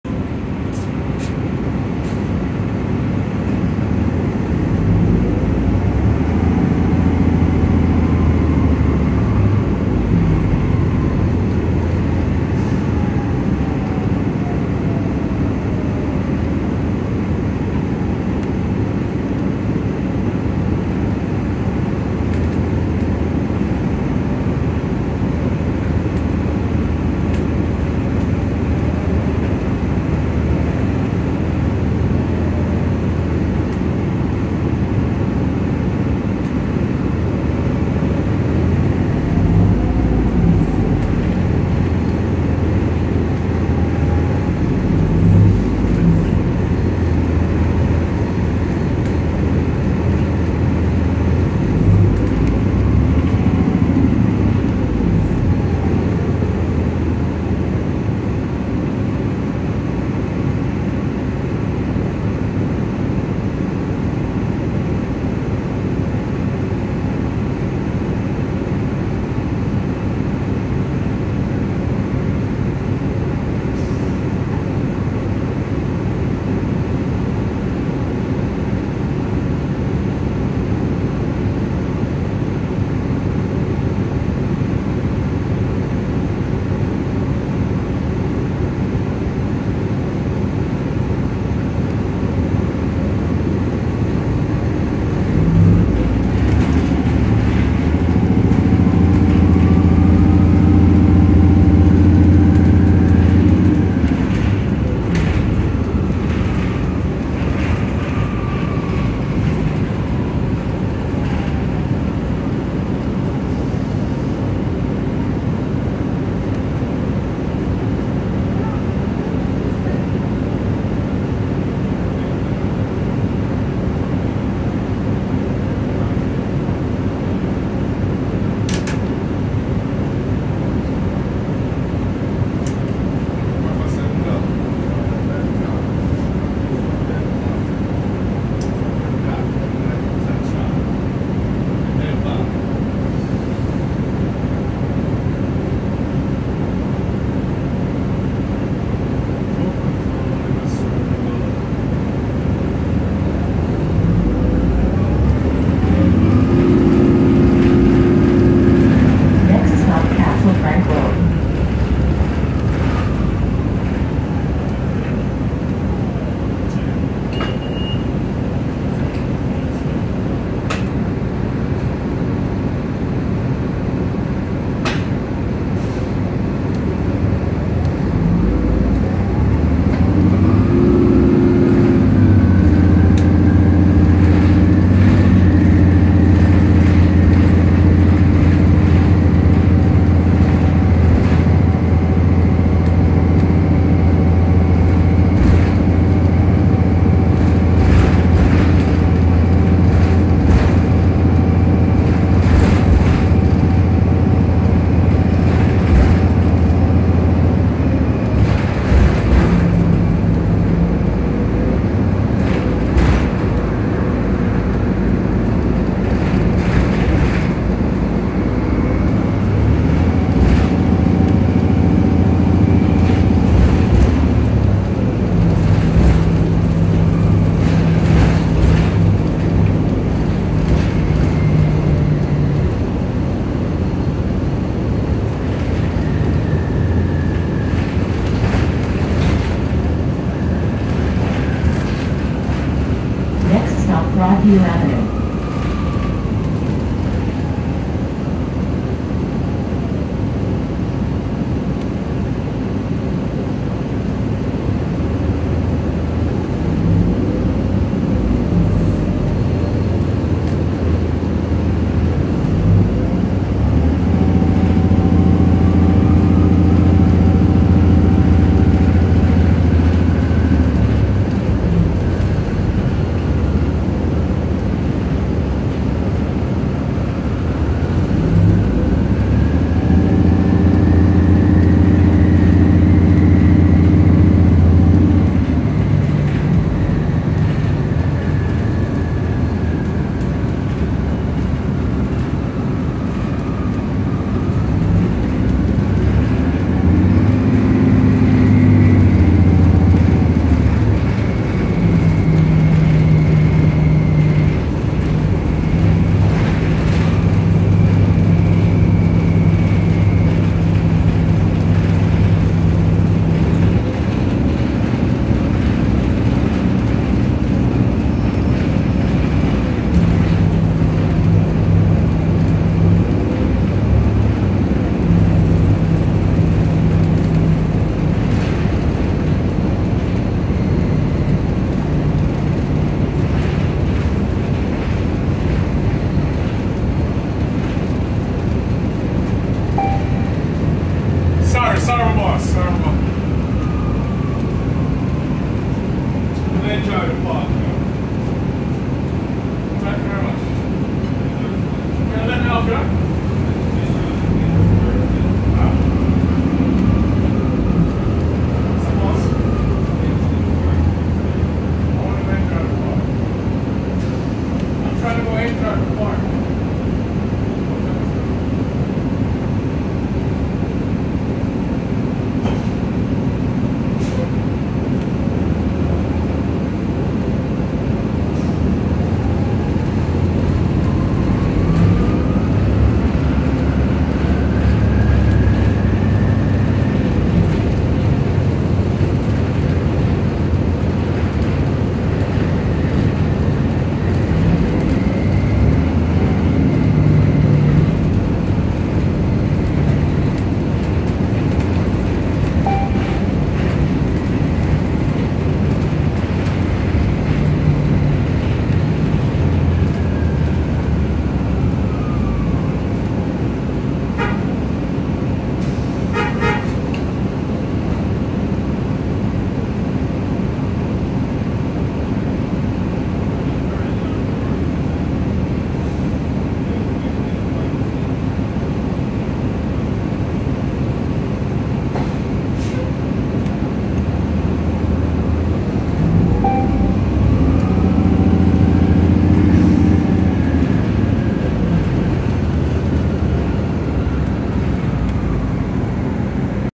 [DETOUR] TTC #1248 | 504/505 King/Dundas Replacement Bus
Toronto Transit Commission 2008 Orion VII NG HEV on the 504/505 King/Dundas Replacement Bus between Castle Frank Station and Gerrard Street East (Hennick Bridgepoint Hospital)